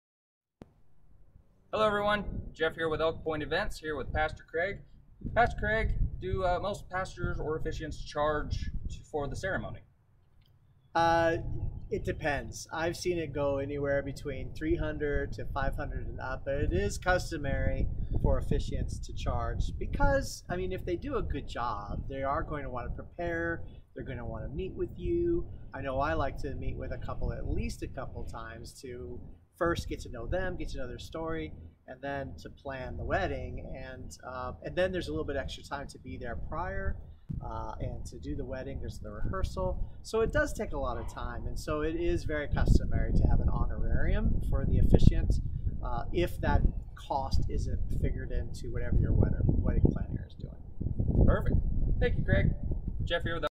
Do Officiants/Minister's charge a fee for their services?
Join Jeff and Pastor Craig as they provide some information to our viewers of how to plan and budget for the services of a minister or officiant for their wedding.